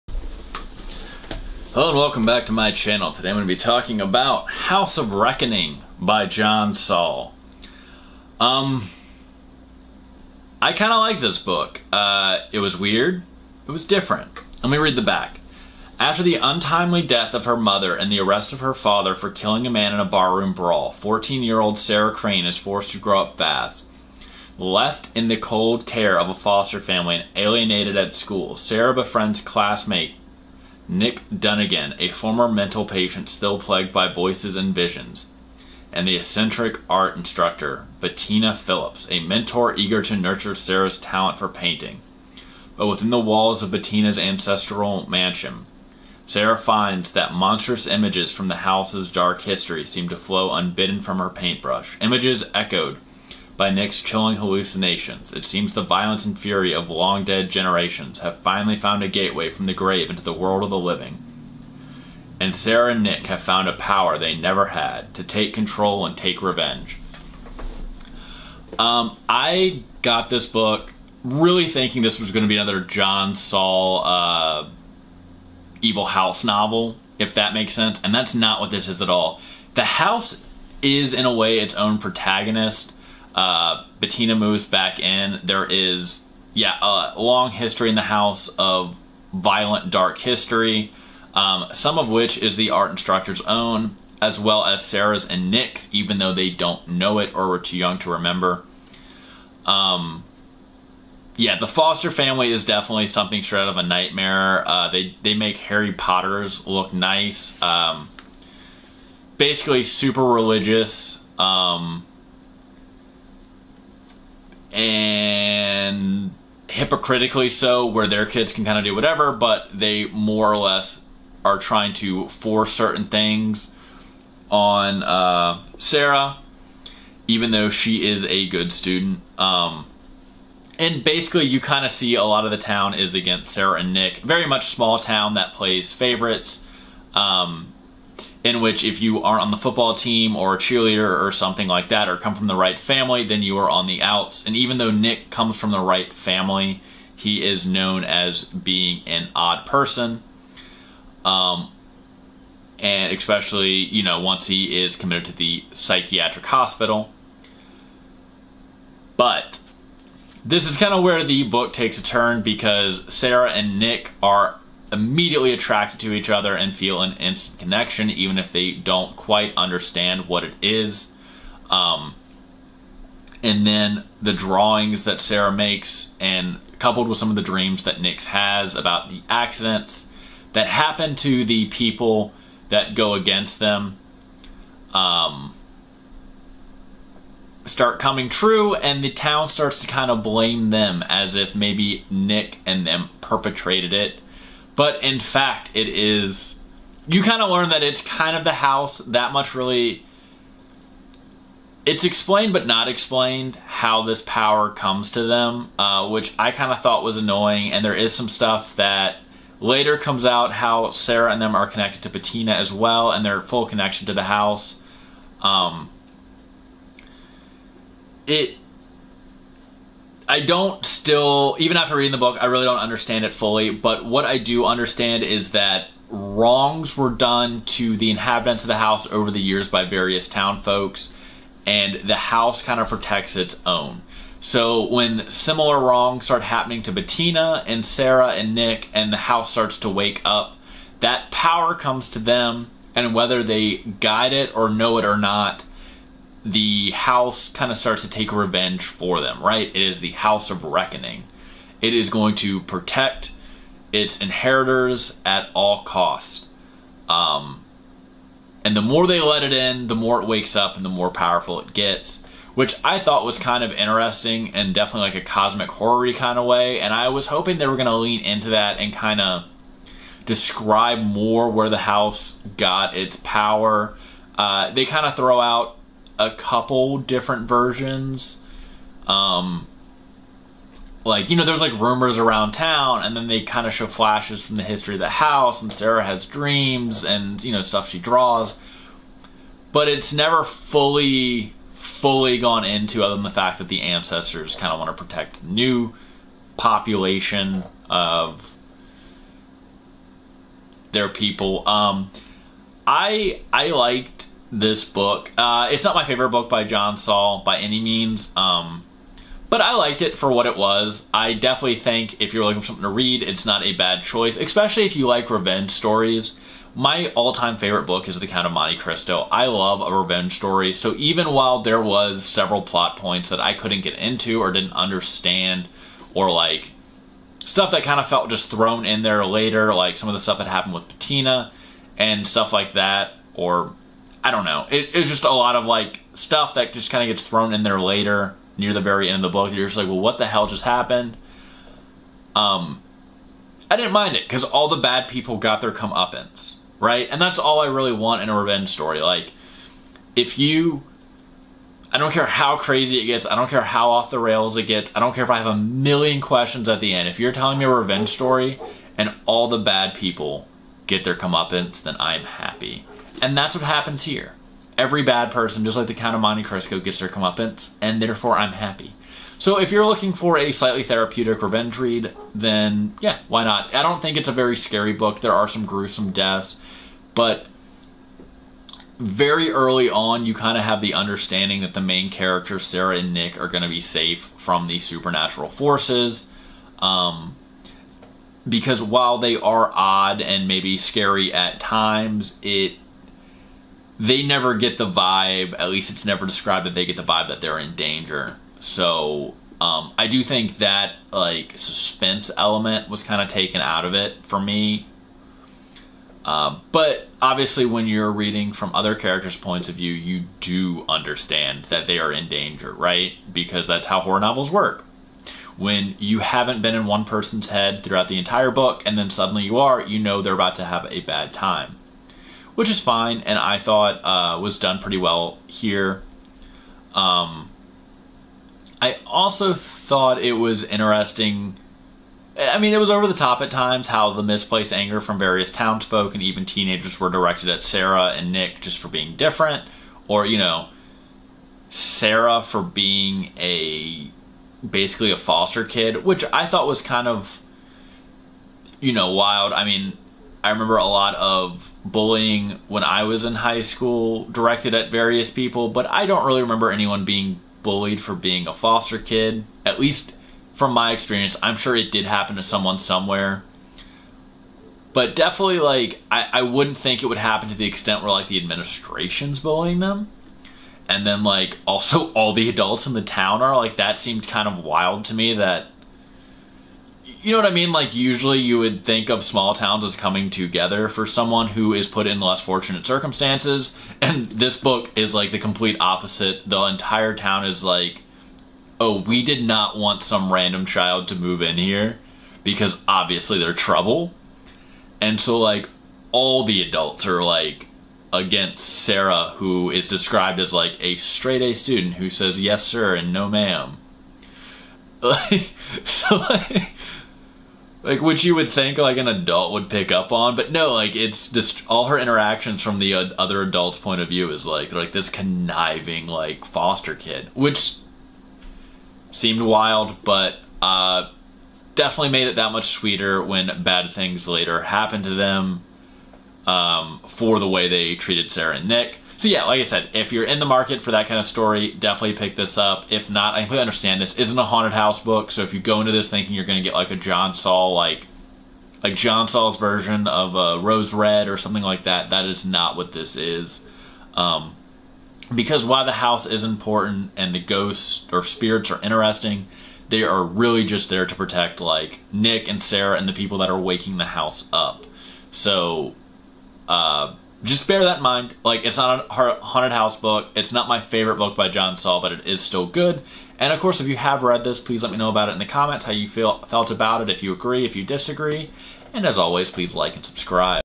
House of Reckoning by John Saul | Book Review
In this video I talk about John Saul's haunted house novel House of Reckoning.